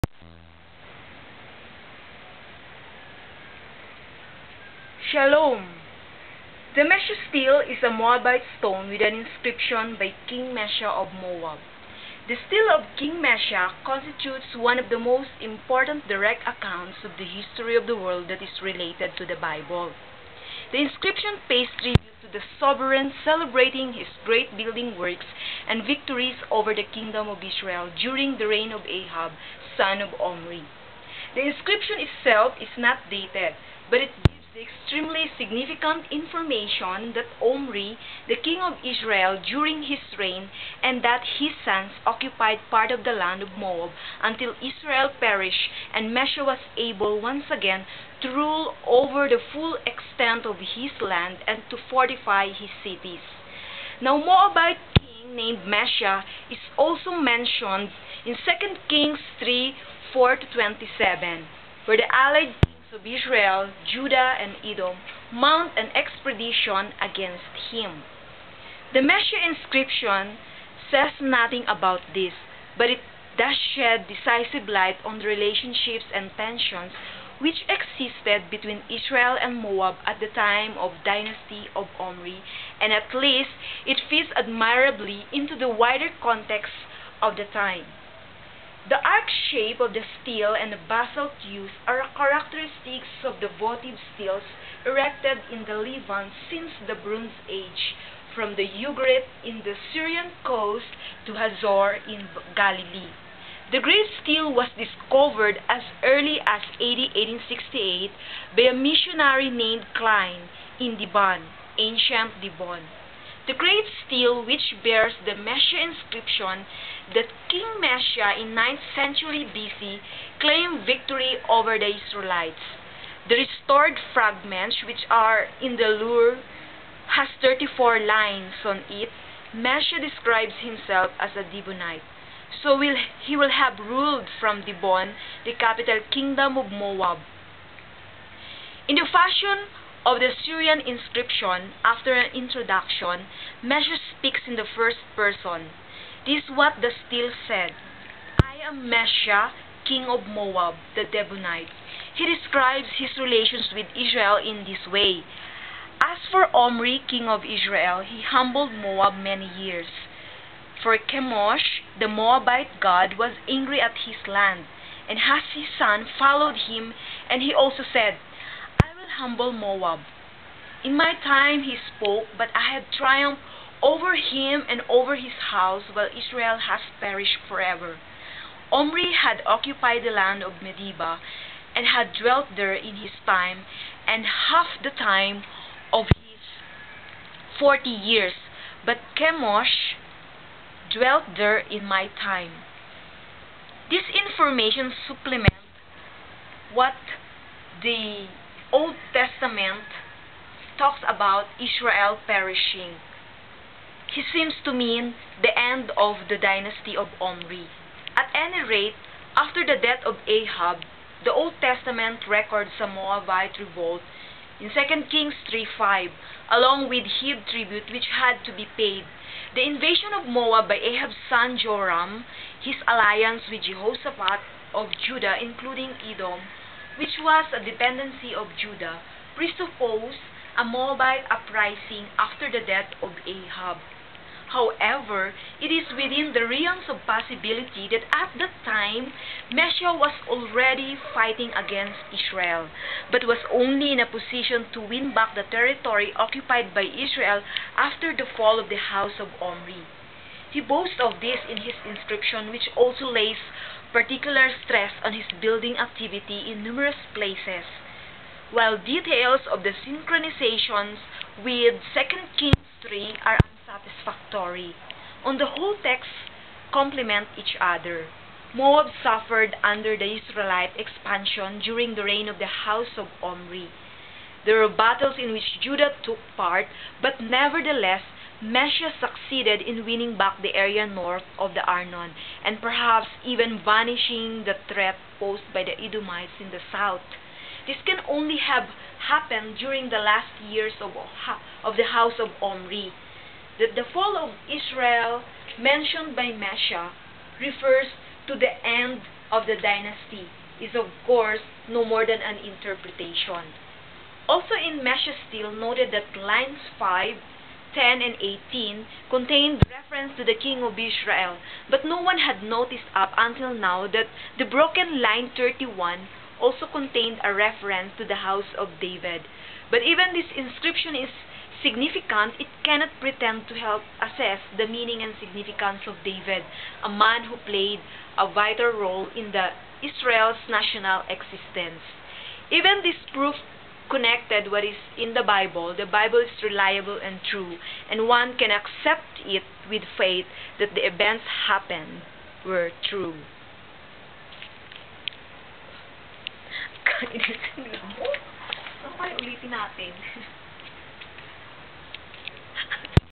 OT 151 - MESHA STELE
This is King Mesha of Moab's inscription. This was discovered by a Missionary by the name of Klein in Dibon in the year 1868.